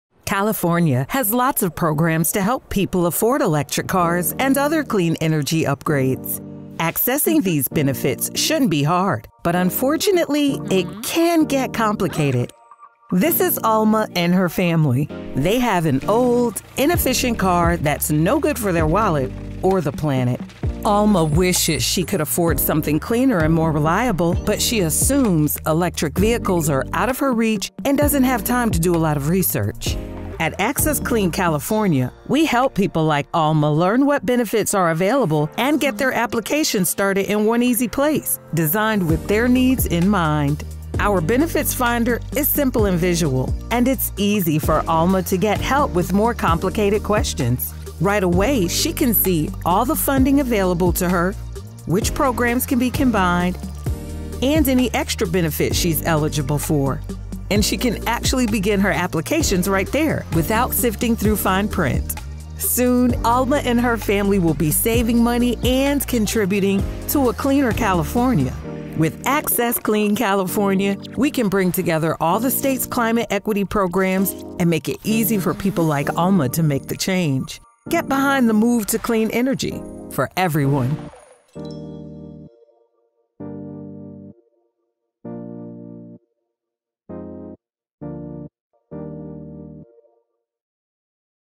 Vidico x GRID Alternatives - Access Clean California Explainer Video
GRID Alternatives is a nonprofit organization that brings the benefits of solar technology to communities that would not otherwise have access.

Access Clean California is a California statewide project that connects income-qualified residents with clean energy and transportation benefits.

Amazing teaming up with them to showcase such an important project.